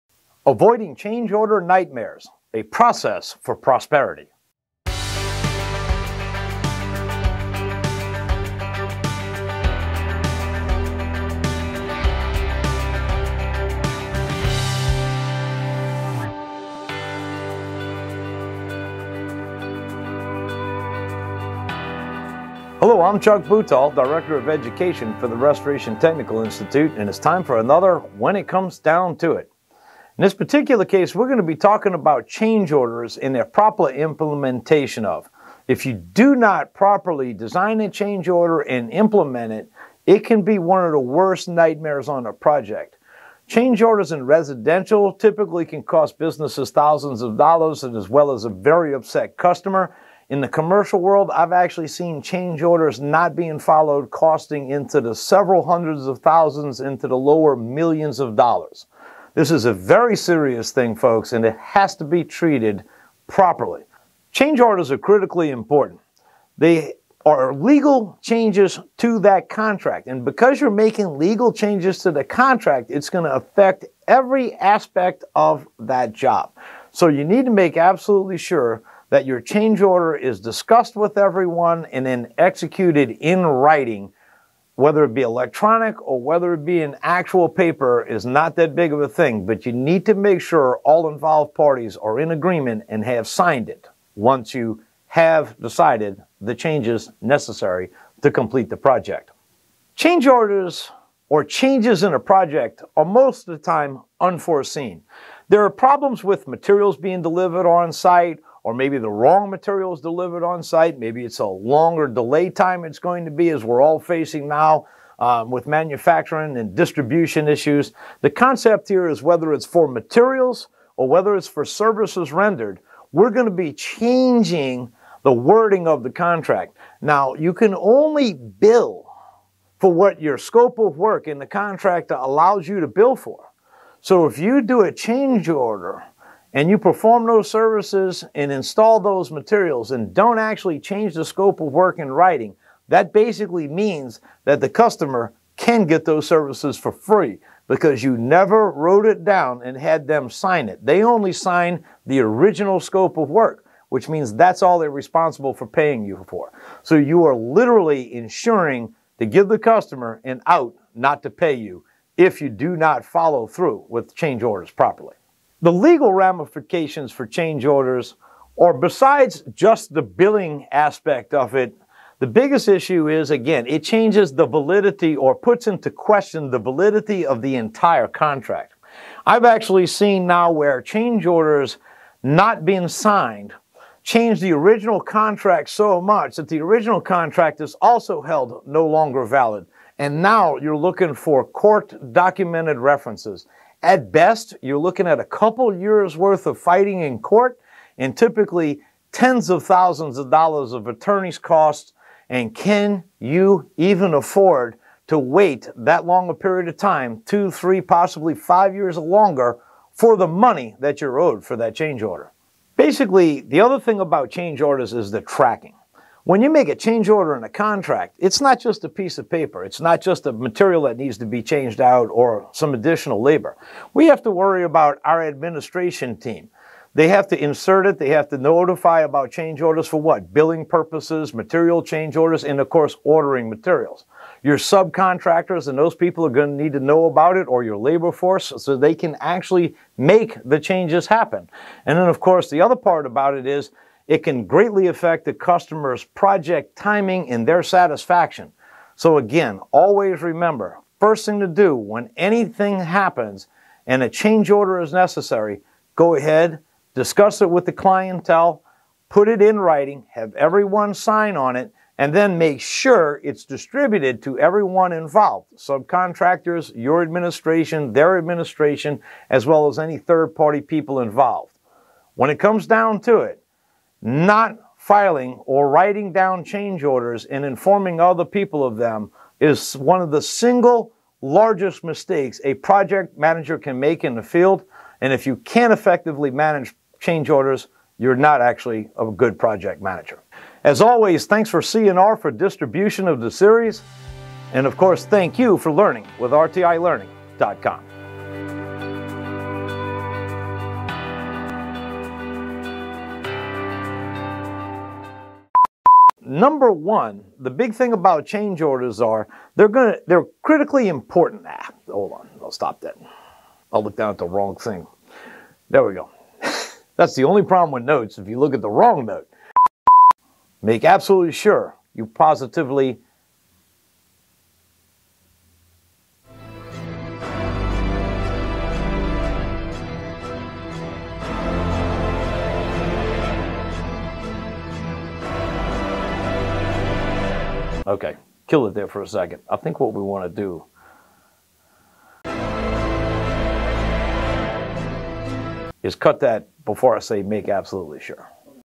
Avoiding Change Order Nightmares
If you're in construction of any kind, it's likely you've faced change order blues.

In this episode of When It Comes Down To It, Chuck walks through legal ramifications of change order mistakes, and the proper steps to take when creating and working through a change order.

Also... be sure you watch to the end for some more great bloopers! You can find these hidden gems on almost every episode with Chuck!

Water damage restoration contractors know that the job comes with many unexpected twists and turns. From hidden water damage to unexpected mold growth, there are many challenges that can arise during the restoration process. One of the most frustrating and costly issues that contractors face is change orders. Change orders occur when there are unexpected changes to the project scope, timeline, or budget. In this video, we will discuss how to avoid change orders during water damage restoration projects.

First, it is essential to establish clear communication with the homeowner or property owner from the outset. It is important to understand their expectations for the project and make sure that you have a thorough understanding of the scope of work. Clearly outline the services that will be provided, the timeline for completion, and the costs associated with the project. This will help to prevent misunderstandings and ensure that everyone is on the same page from the beginning.

Next, it is important to conduct a thorough inspection of the property before beginning the restoration process. This will help you to identify any potential issues that could lead to change orders down the line. Look for signs of hidden water damage, such as moisture buildup or discoloration on walls or ceilings. Use moisture meters and thermal imaging cameras to identify areas of the property that may be prone to mold growth or other issues.

Once you have identified any potential issues, be sure to discuss them with the homeowner or property owner. Provide a detailed explanation of the potential risks and costs associated with each issue, and offer solutions to address the problem. By being transparent and upfront about potential issues, you can avoid surprises down the line and build trust with your clients.

Another key strategy for avoiding change orders is to establish a detailed project plan and timeline. This should include all of the tasks that need to be completed, along with deadlines for each task. Be sure to build in contingency time for unexpected issues or delays. By creating a detailed plan and timeline, you can help to ensure that the project stays on track and that unexpected issues are kept to a minimum.

Finally, it is important to work with reputable suppliers and subcontractors. Make sure that all of your suppliers and subcontractors are licensed, insured, and experienced in water damage restoration. This will help to ensure that the work is completed to a high standard and that any issues can be addressed quickly and effectively.

In conclusion, water damage restoration projects can be complex and challenging, but by following these tips, you can help to minimize the risk of change orders and ensure that the project is completed to a high standard. By establishing clear communication with the homeowner or property owner, conducting a thorough inspection, creating a detailed project plan and timeline, and working with reputable suppliers and subcontractors, you can help to ensure the success of your water damage restoration project.